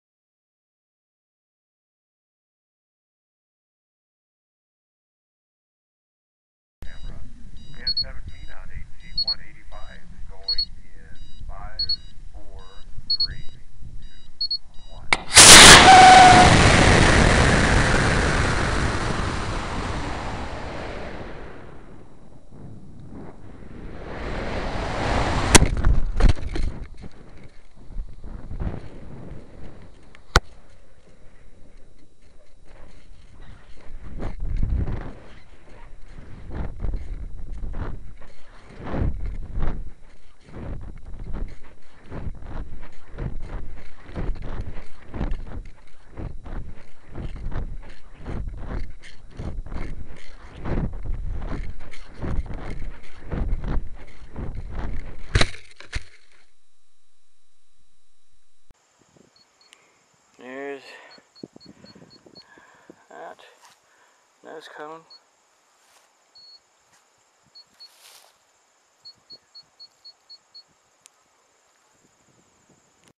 Flight of the Werewolf Killer
Onboard video of an original design rocket built with a 4" Bluetube frame, a LOC nose cone, and CNC-cut fins. Designed to fly on mid power to low high power motors. With a more versatile 38mm motor mount it flies very well on the CTI 1-grain G motors.